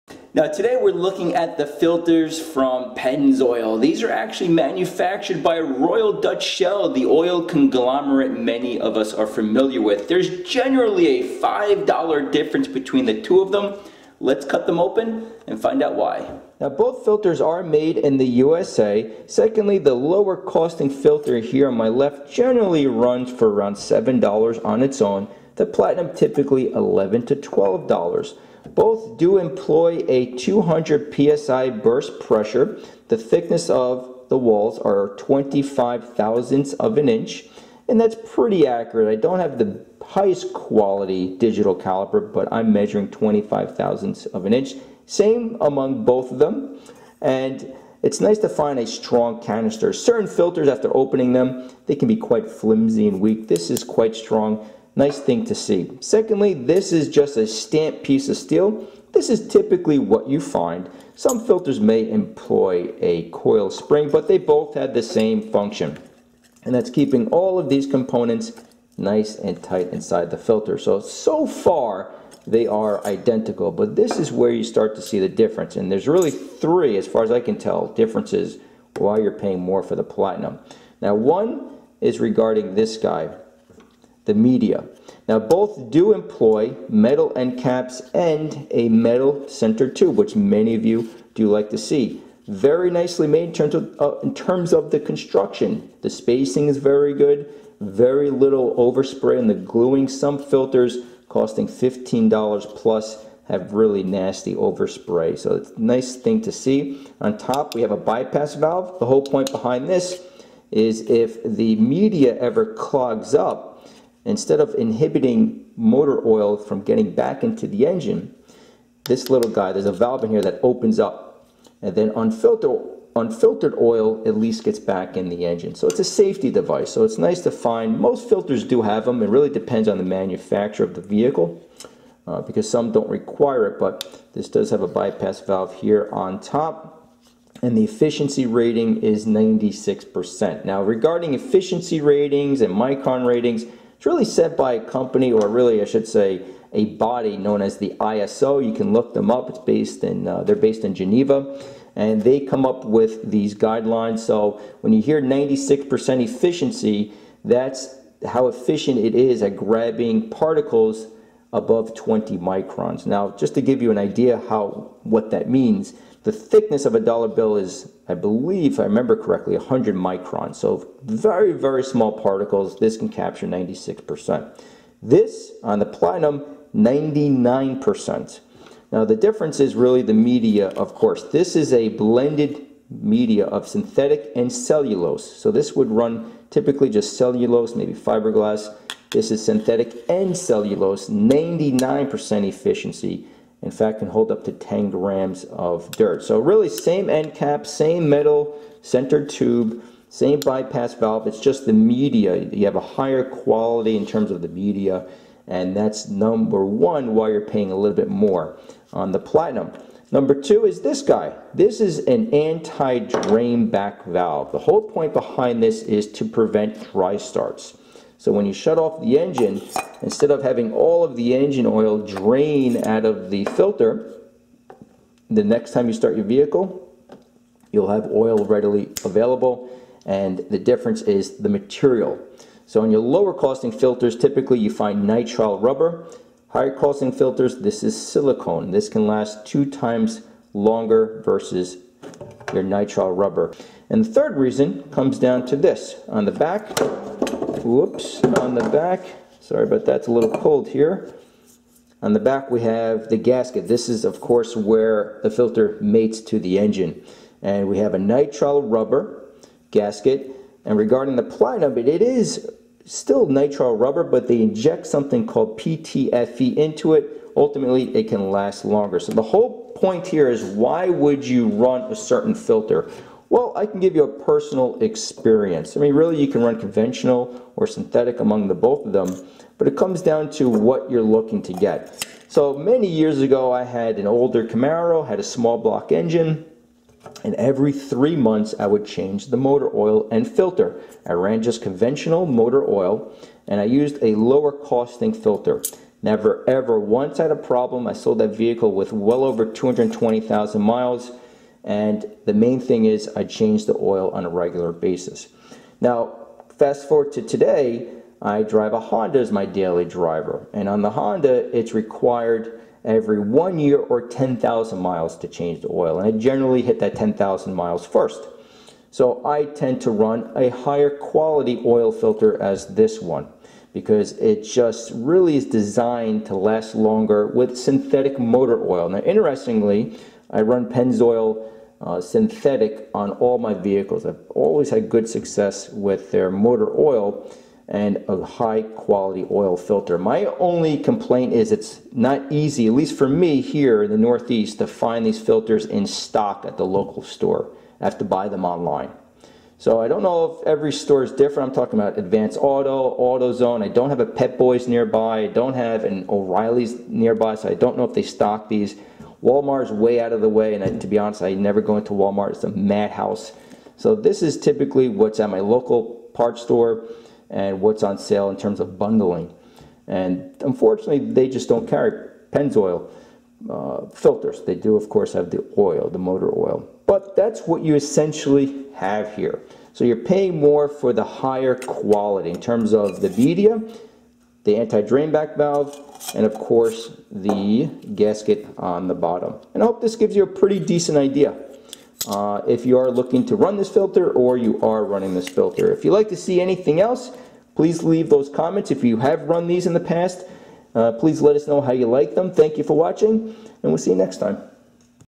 Pennzoil Oil Filter Comparison | Regular Spin-On vs Platinum HE
Today we're looking at Pennzoil's Regular Spin-On filter vs their Platinum HE filter. We go over the burst pressure, thickness of the canister, the filter media (cellulose vs synthetic blend), efficiency ratings, bypass valve, anti-drainback valve and the gasket material.

►► Tools and Supplies:
Pennzoil PZ-37
Pennzoil HPZ-37
Safety Glasses